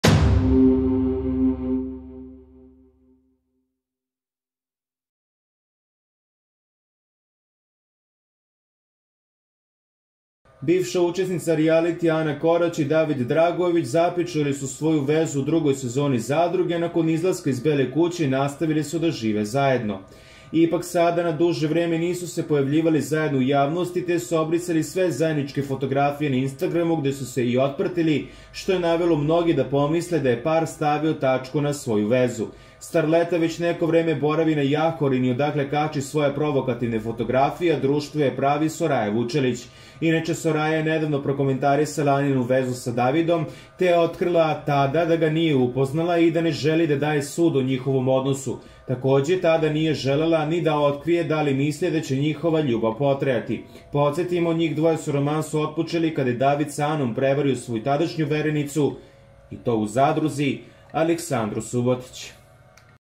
Sada je SVE jasno: Evo sta je Ana Korac URADILA nakon sto je SUTNULA Davida! Au
Bivši učesnica rijalitija Ana Korać i David Dragojević započeli su svoju vezu u drugoj sezoni "Zadruge", a nakon izlaska iz Bele kuće nasavili su da žive zajedno.